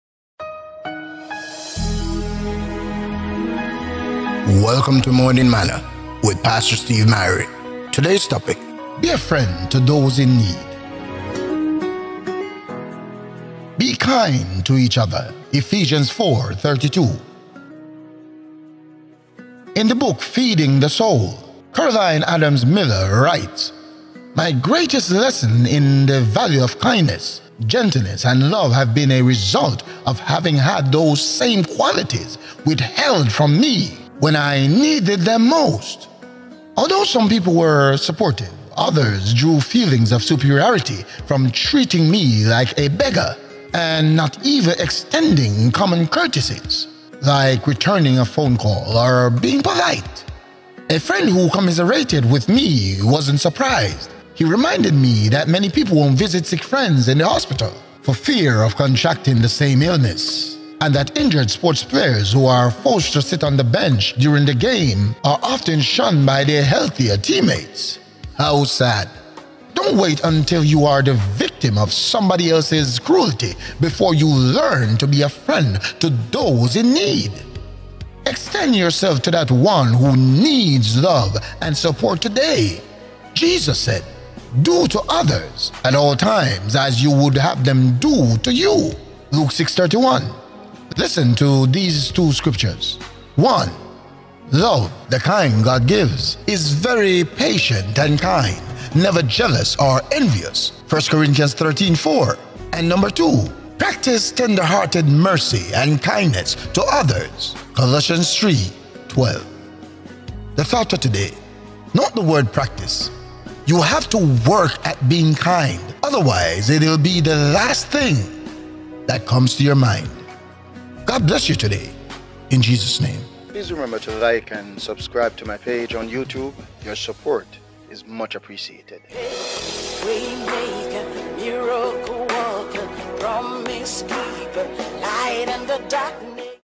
Morning Morning Season 2 - Episode # 250 Topic : Be a Friend to Those in Need
Be kind to each other. (Ephesians 4:32 TLB)